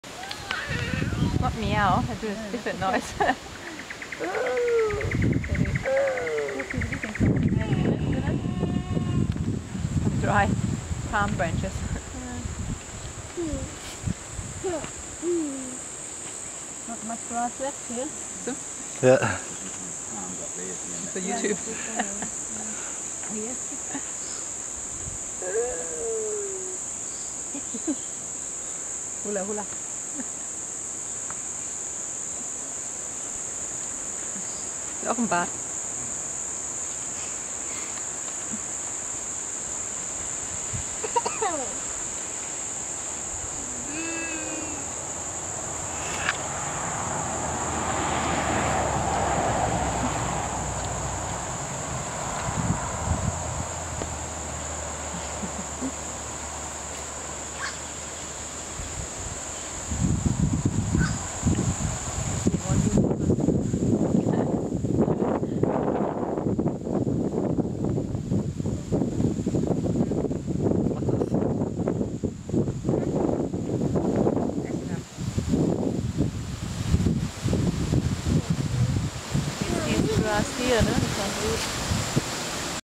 camel movie
Those camels were filmed at the South East of Queensland near the Sunshine coast.
They are used for making milk and are very close to our home.